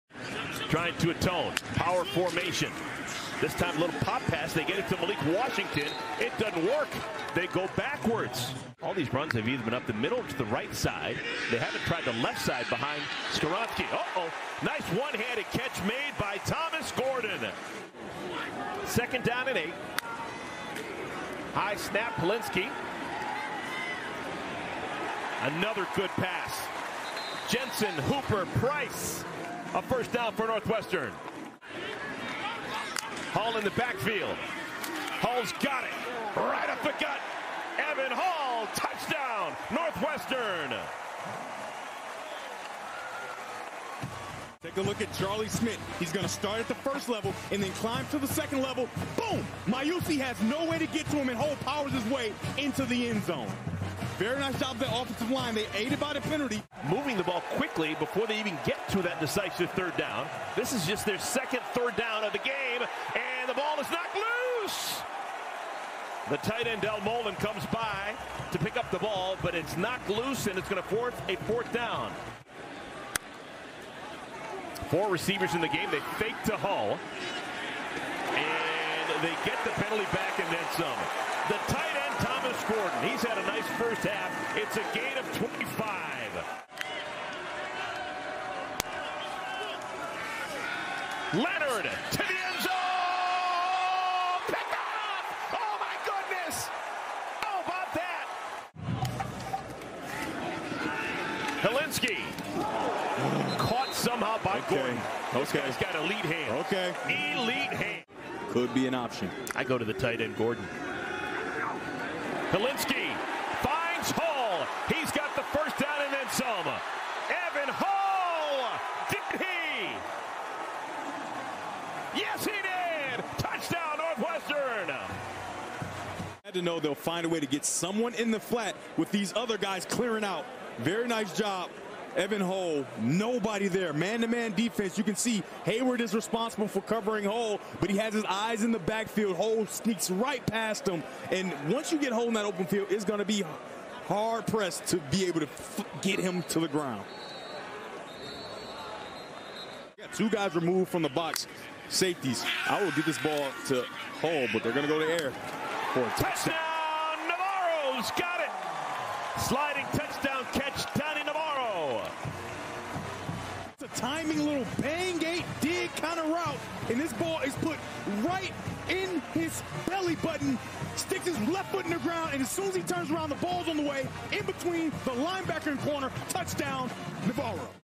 Northwestern Football - Duke Highlights (9/10/2022)
Catch the Northwestern Highlights against Duke on September 10th, 2022.